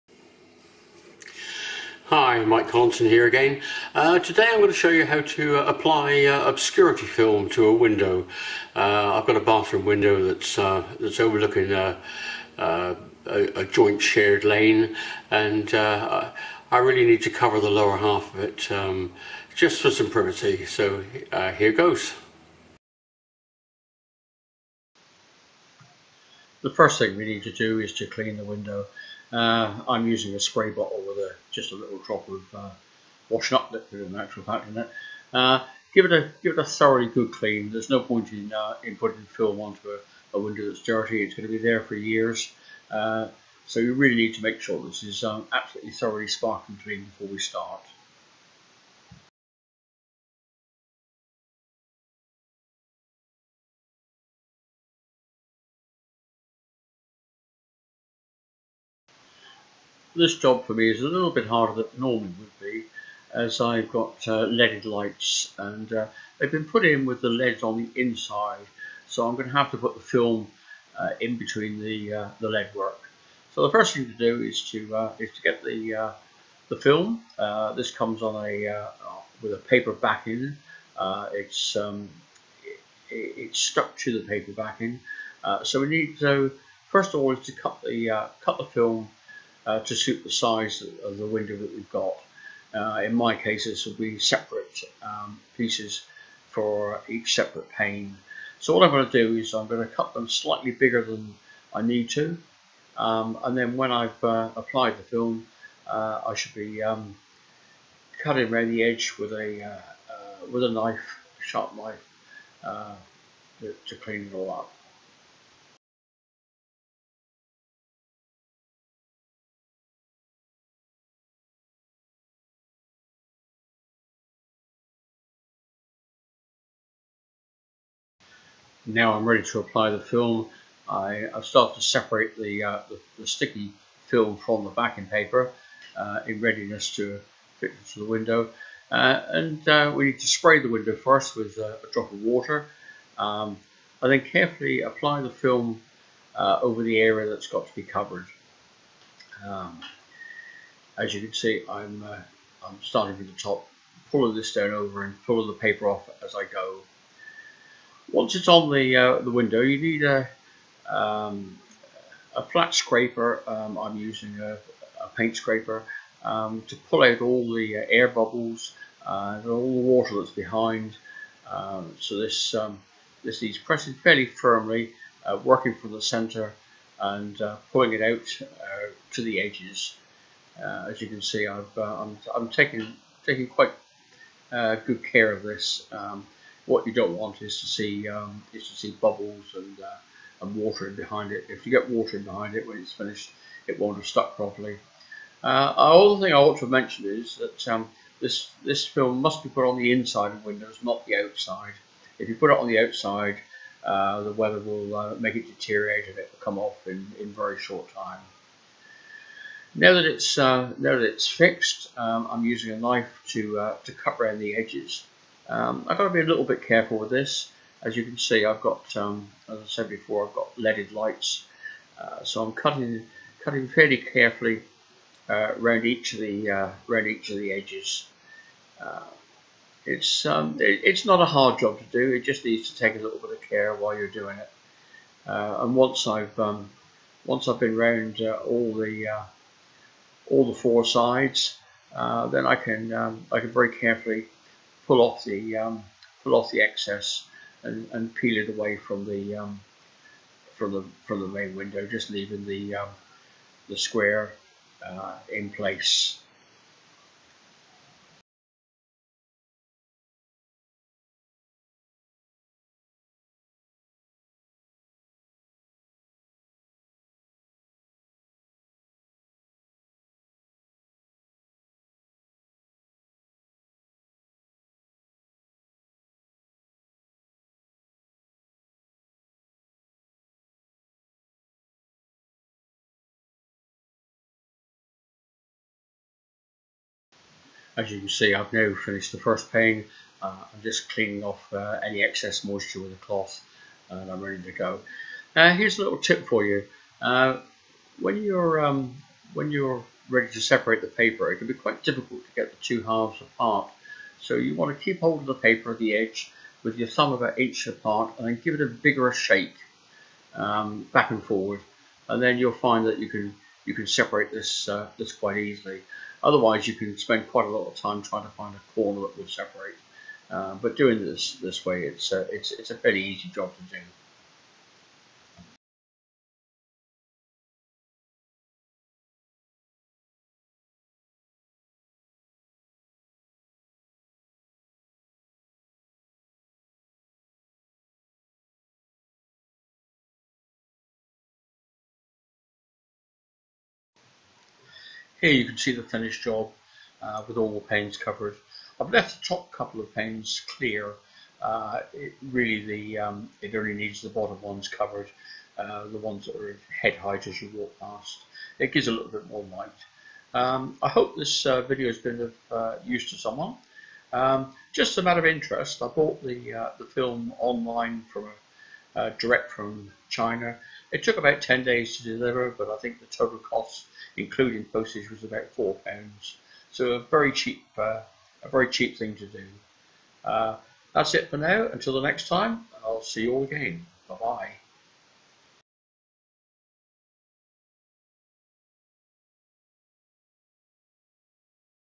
Applying Window Film
How to apply self-adhesive frosted privacy film to a window, here, I am showing how to apply the film to a window, the most important thing is to make sure the glass is absolutely clean before starting. Lightly spray the glass with water, apply the film and spread out from the centre with a spatula.